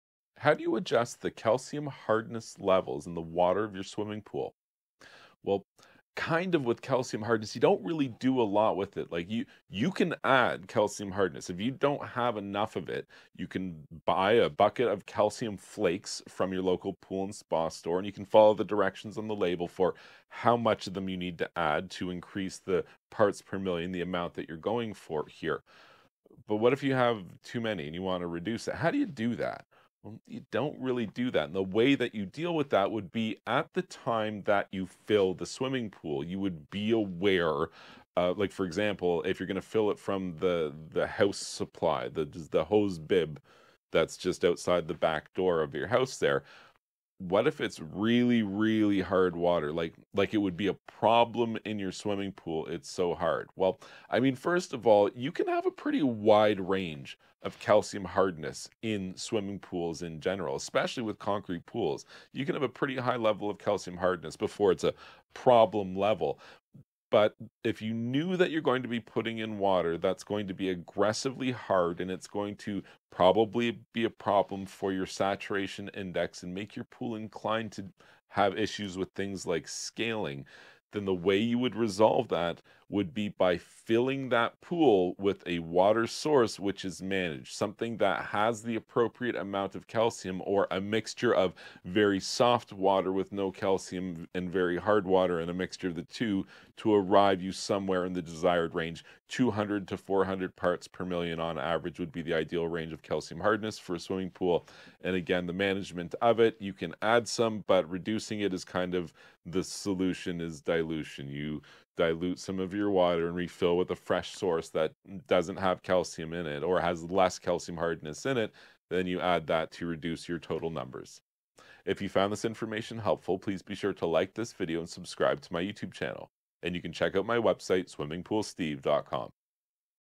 How To Adjust CALCIUM HARDNESS In Pool Water
this video talks about one of the lesser common pool chemistry variables that you might need to adjust in your pool water - calcium hardness.  Calcium hardness is the measurement of the amount of calcium and magnesium in your pool water and should be in the 200 ppm to 400 ppm range for most swimming pools.

To raise calcium hardness you purchase calcium flakes from a pool and spa store and dissolve them in a bucket of pool water before adding to the pool.  Always follow the instructions for the product you are using.  Every pool chemical sold comes with instructions on the label for how to use, and how much to use in your pool to achieve the desired change in water chemistry.  Be aware that calcium generates heat when you mix it with water so never mix with your hands, and always store calcium hardness in a secure, dry area.

To lower calcium hardness in your water requires dilution of your pool water with fresh water than has less calcium hardness.  If you use cal-hypo type of pool chlorine this will leave a calcium residual in the water which is how your calcium hardness levels can climb over time.